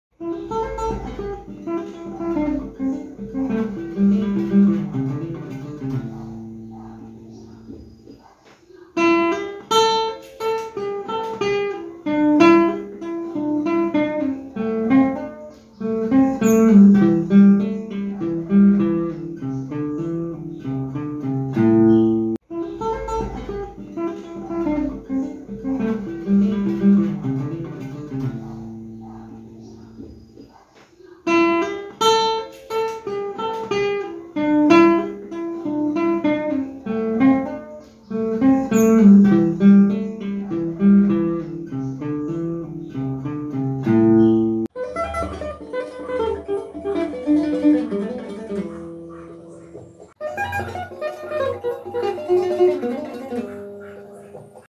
EXERCÍCIO SOBRE A ESCALA PENTATÔNICA DE LÁ MENOR | EXERCÍCIO ESCALA PENTATÔNICA NO VIOLÃO
Escala pentatônica para violão e guitarra em lá menor. Exercício técnico sobre a escala pentatônica. Breve estarei fazendo um estudo completo no canal.

Você terá acesso ao arquivo em alta resolução para elaborar o adesivo e preparar o seu quadro. Se você tem dúvidas, assista a este vídeo